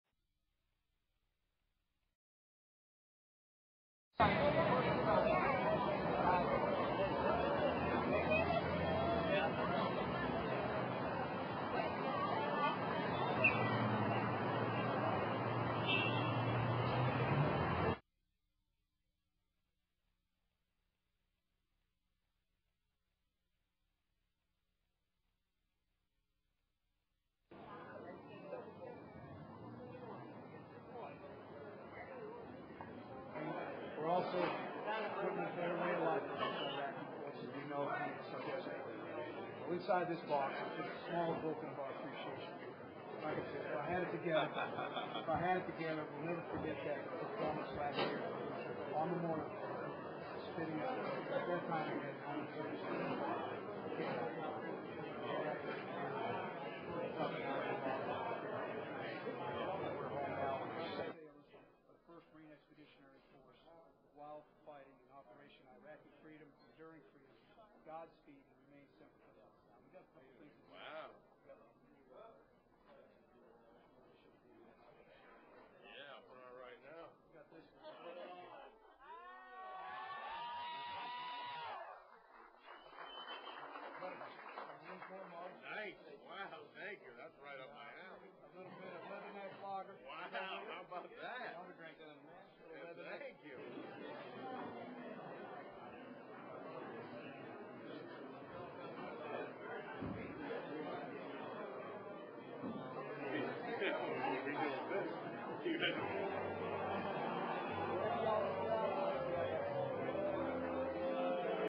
Marines on Jimmy Kimmel Live
Camp Pendleton Marines attending a taping of Jimmy Kimmel Live. Scenes include Marines meeting Jimmy Kimmel and Toby Keith and presenting gifts to both men.